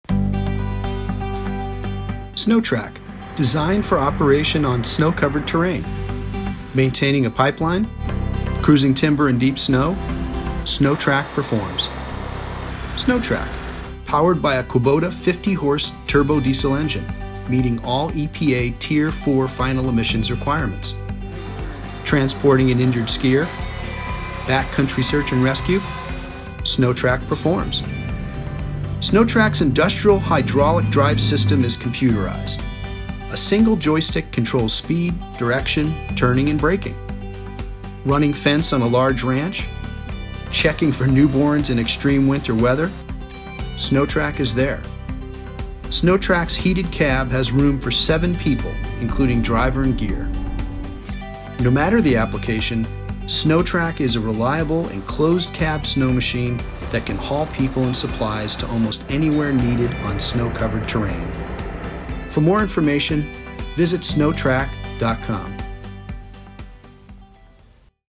Sno Trac: Made for Adventure, Built to Last
Sno Trac is the most affordable American made snow vehicle that can be used to transport up to 7 passengers in a heated cab. The new and improved Sno Trac is an easy to operate, tough, off-road, tracked vehicle. It utilizes a single joystick that controls speed, direction, turning and braking.

DOMINATE THE SNOW IN ANY CONDITION
This machine is great for outer adventurers, 4x4 enthusiasts, ice fisherman, ski lodge establishments, farmers and owners of mountain cabins and lodges. Although they are designed to operation on snow, they may be used on most of the earth's toughest terrains.

MADE FOR PROFESSIONAL & RECREATIONAL PURPOSES
Our Sno Tracs can be used for recreation, pipeline maintenance, cell tower access, search & rescue, ranching, surveying, ice fishing, transportation, energy exploration, and much more.
COLOR OPTIONS ARE ENDLESS
While there are seven standard color options, you can buy a Sno Trac in any color you desire.

Overall Dimensions: 12’ (3600 mm) long – 6’ 3” (1875 mm) wide
Heated Cab seats up to 7, including driver
49.6 hp. Kubota Turbo Diesel engine – EPA Tier IV Final Emissions Compliant
Closed Loop hydrostatic drive